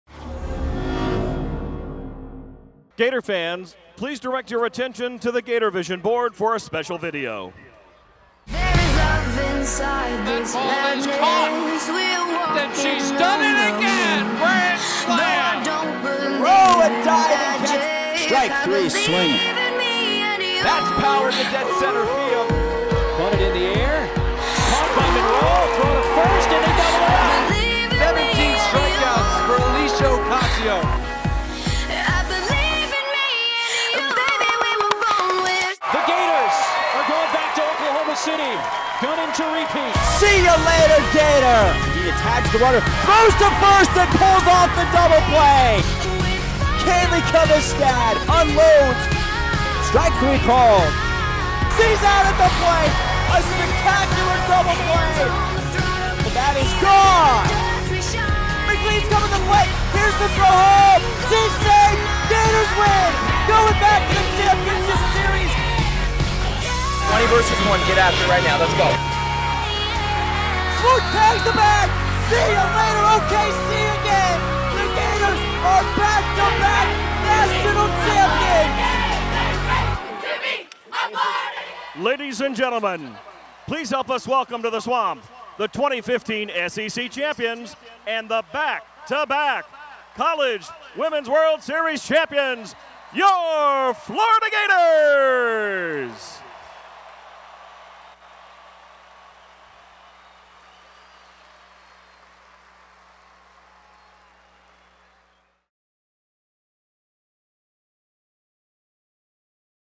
Florida Softball: On-Field Recognition 9-26-15
The 2015 Florida softball team was recognized at The Swamp during the Florida vs. Tennessee football game for their SEC & NCAA Championships!